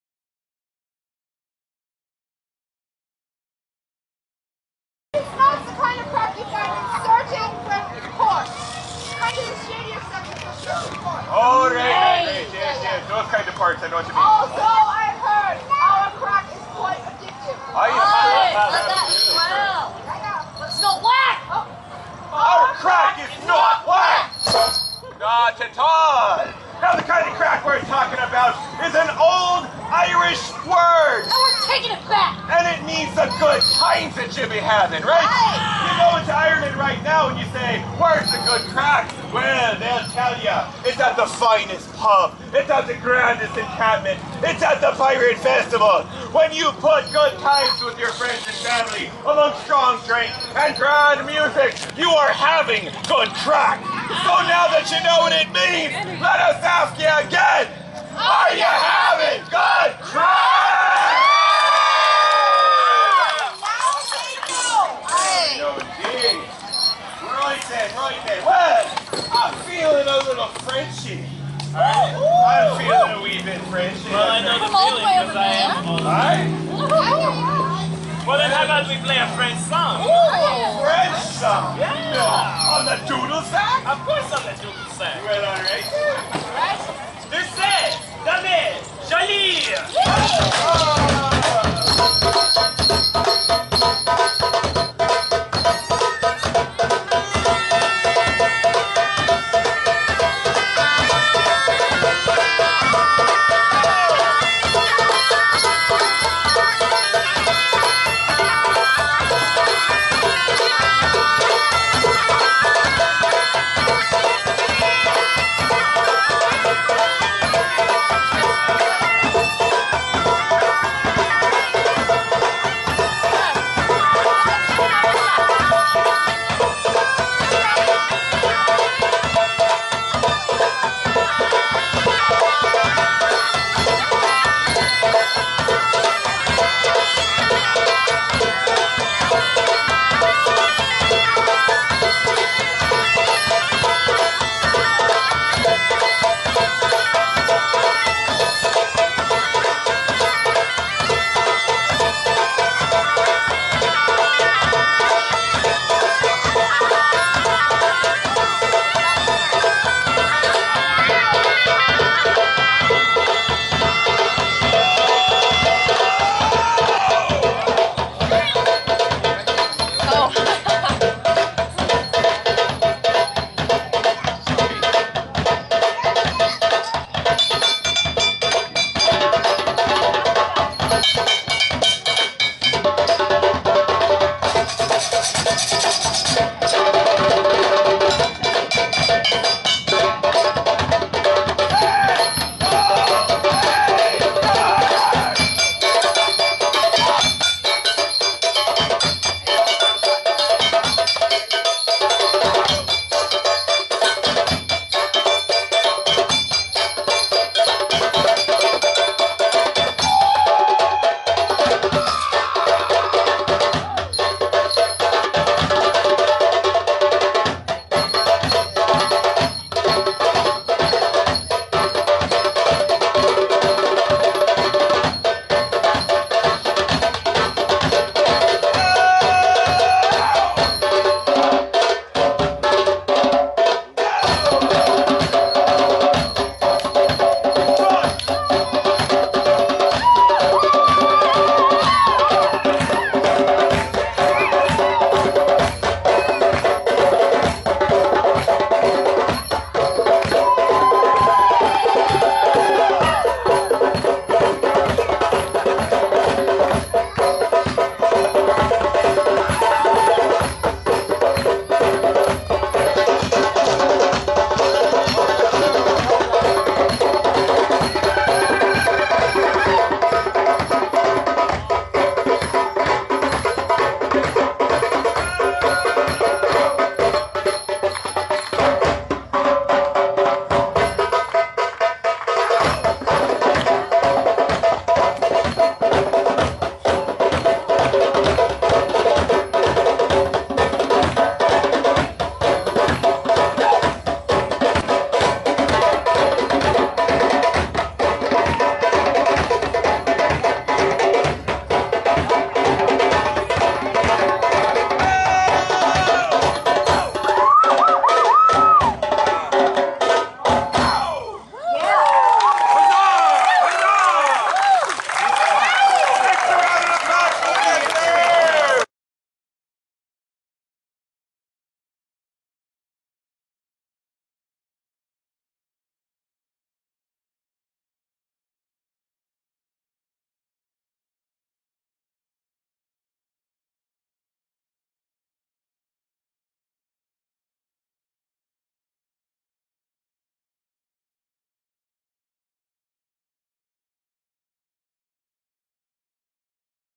Craic at the Black Hearted Tavern Feb 2014
The Craic Play @ the Black Hearted Tavern during the 2014 Treasure Coast Pirate Fest to visitors & Pirate en campers alike.